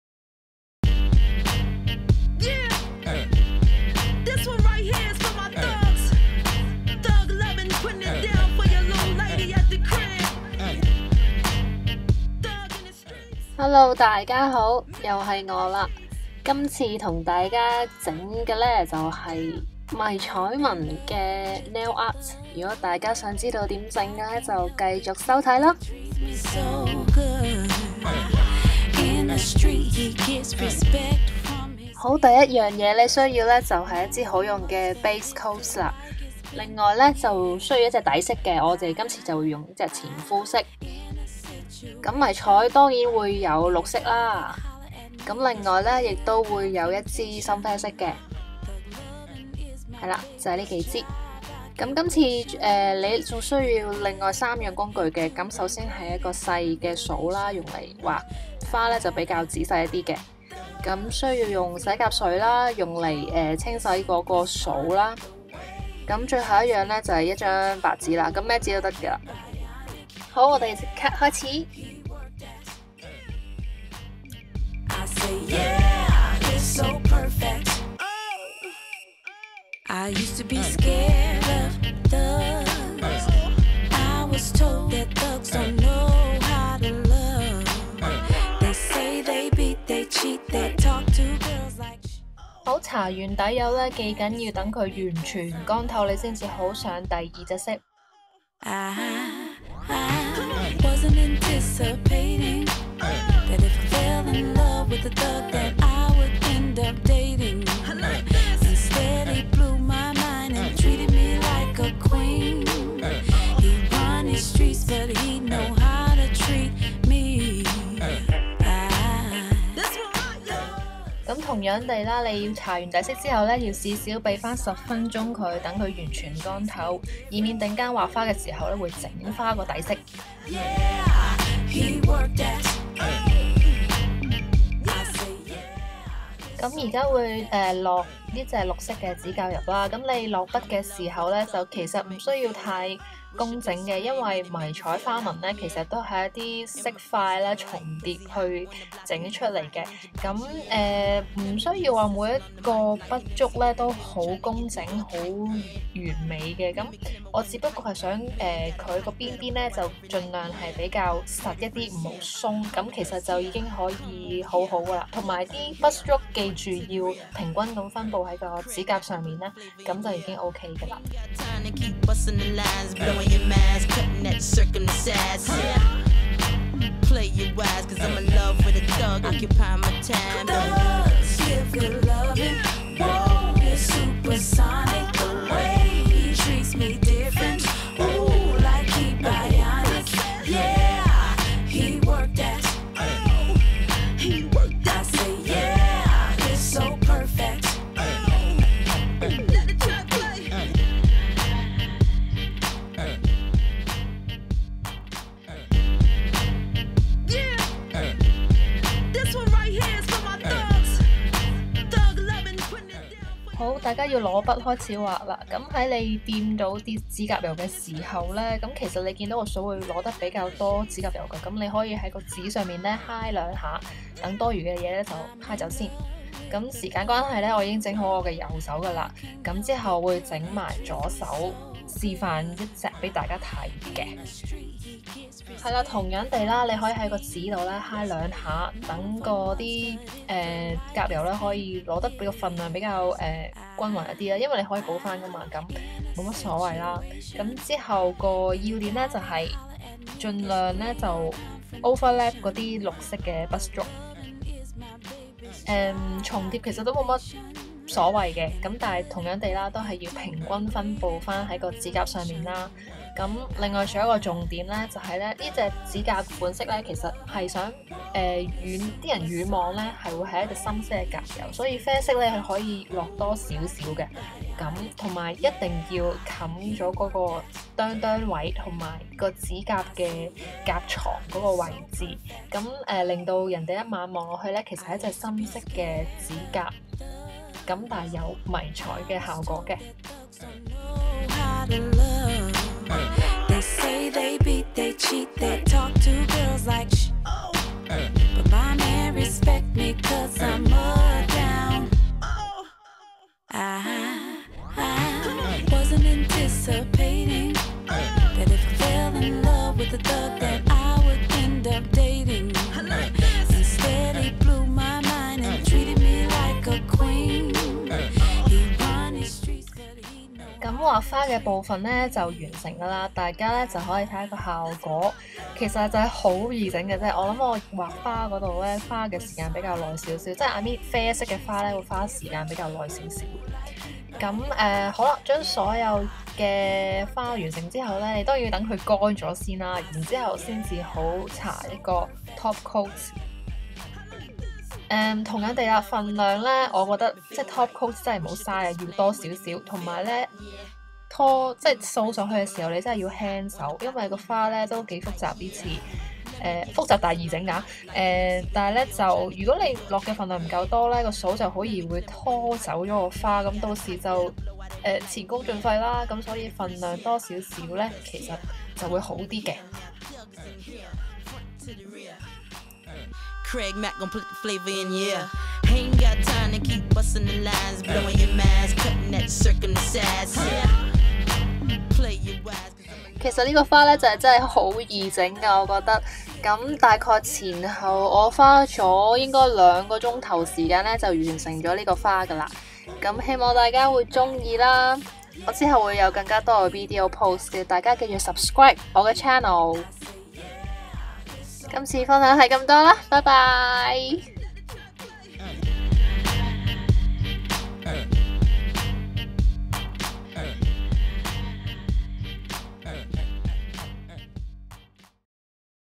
Nail : 迷上身，迷彩教學 camouflage nail tutorial
EXPAND TO READ ^^

迷彩放上身...

Preparation & Tools:
1) Priti NYC 2-in-1 top & base coat
2) Essie nail polish in 304Jazz
3) Essie nail polish in #734 Going incognito
4) Revlon nail enamel in #903 Hot for chocolate
5) Small tip nail brush
6) Nail polish remover
7) Plain worksheet

Watch video for detail step by step tutorial.